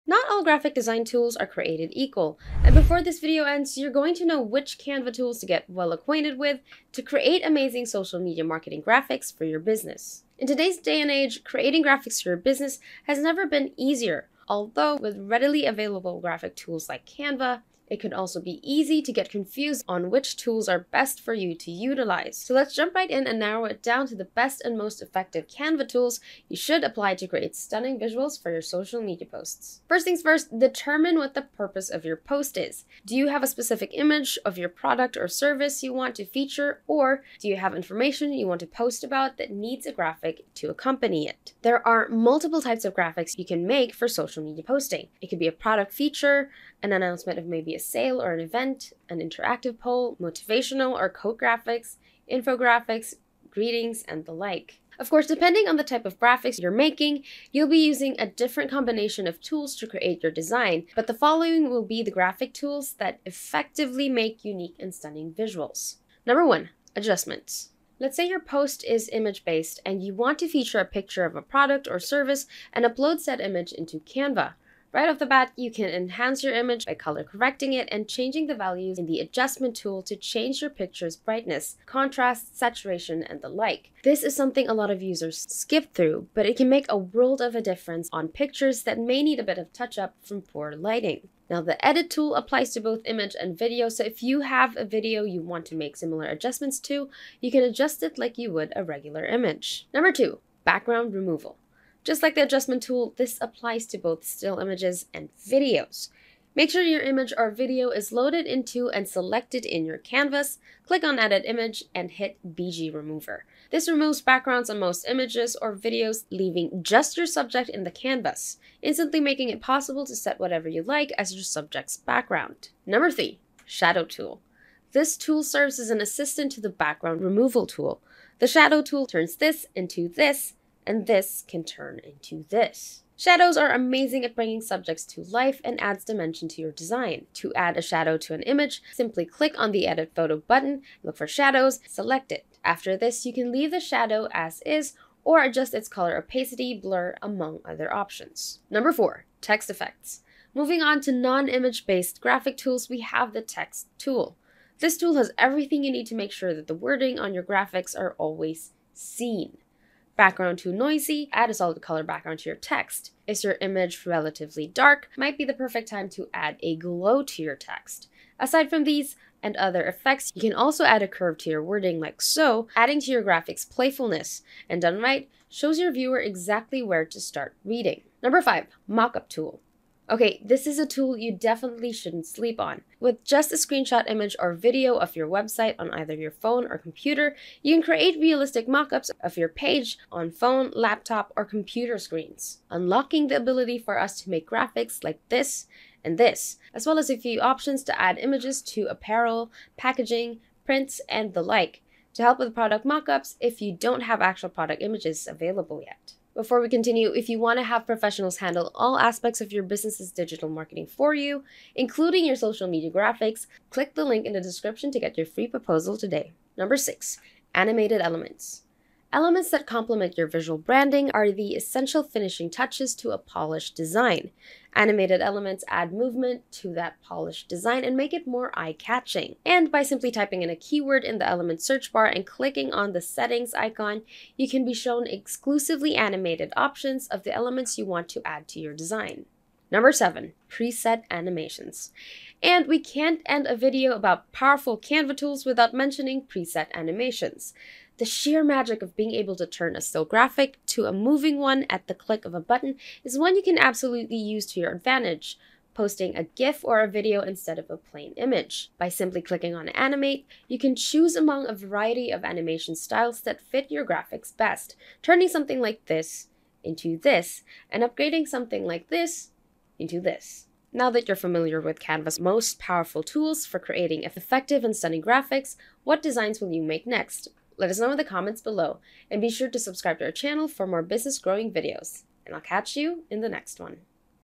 THESE Canva Tools Will Make Your Social Media Images BETTER
Watch this video to find out which Canva tools to use to create amazing social media marketing graphics for your business!

Not all graphics design tools are created equal and before this video ends, you’re going to know which Canva tools to get well acquainted with to create amazing social media marketing graphics for your business.

In today’s day and age creating graphics for your business has never been easier, although with the readily available graphics tools on design sites like Canva it could also be easy to get confused on what tools are best for you to utilize. So let’s jump right in and narrow it down to the best and most effective Canva tools you should apply to create stunning visuals for your social media posts.

Video Outline
Intro (0:00)
#1 - Adjustments (1:07)
#2 - Background Removal (1:40)
#3 - Shadow Tool (2:04)
#4 - Text Effects (2:29)
#5 - Mockup Tool (2:59)
#6 - Animated Elements (3:38)
#7 - Preset Animations (4:01)

// LYFE Marketing